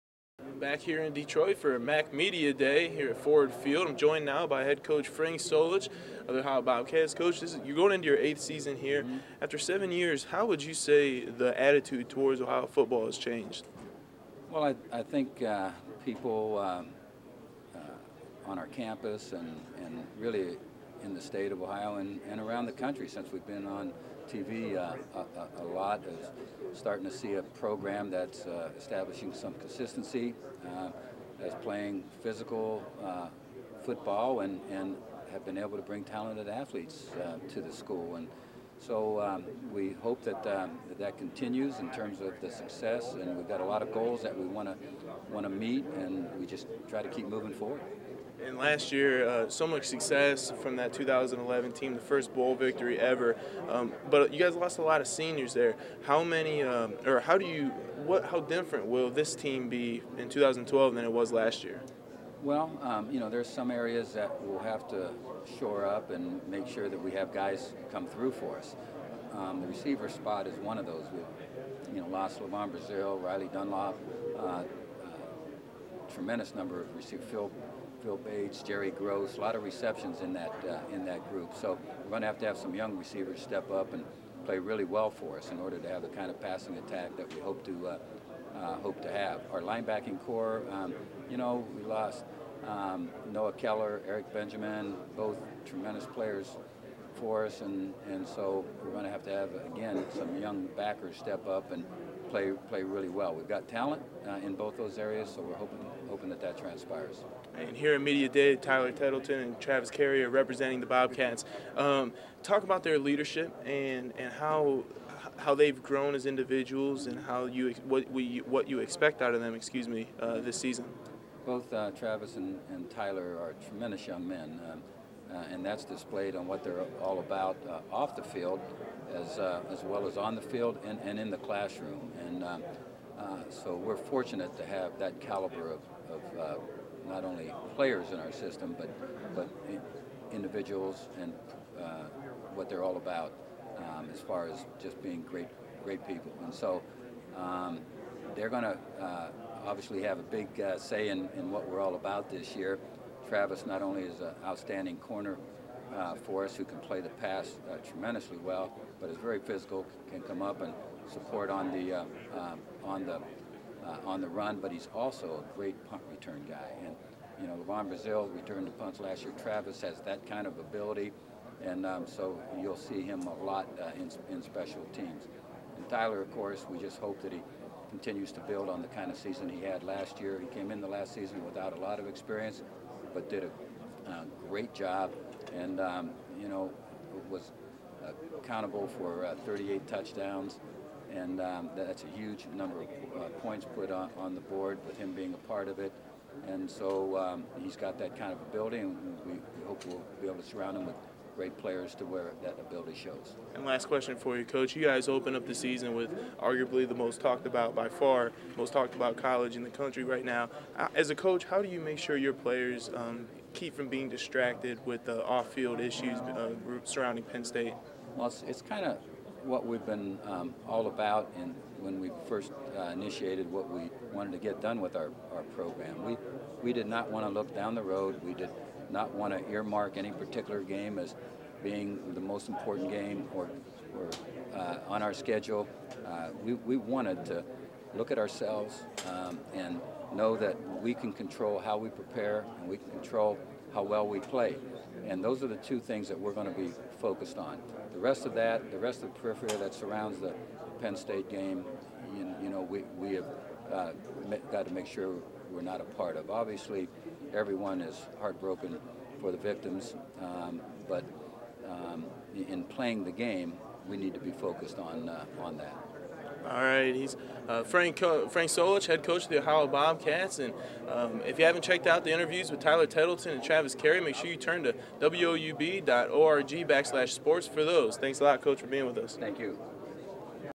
MAC Football Media Day: Coach Frank Solich
WOUB and Bobcat Sports Showcase reporter Mark Pierce sits down with Ohio head football coach Frank Solich at the 2012 MAC Football Media Day at Ford Field in Detroit, MI.